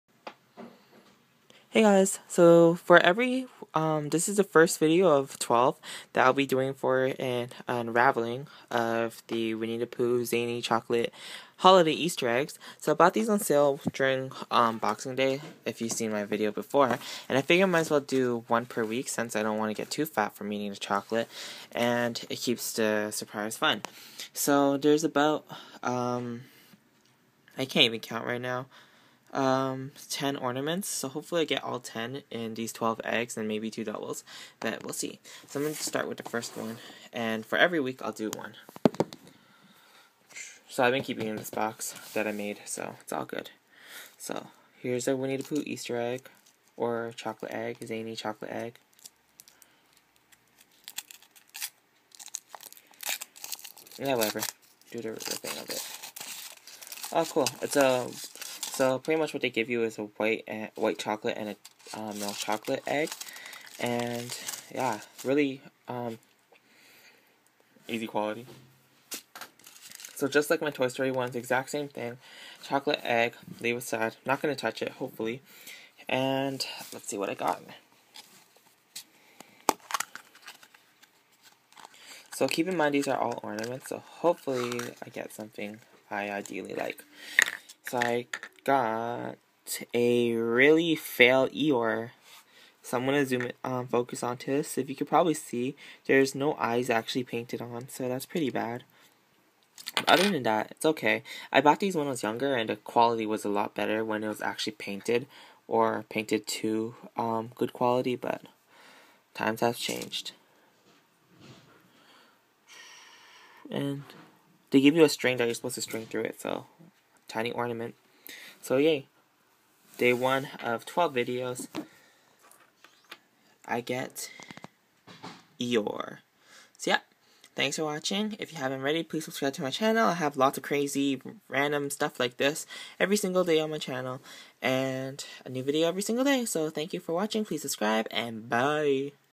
Surprise Eggs Unboxing Kinder Surprise Zaini Winnie the Pooh (1 of 12 Eggs)
A quick look at the Winnie the Pooh inspired chocolate egg.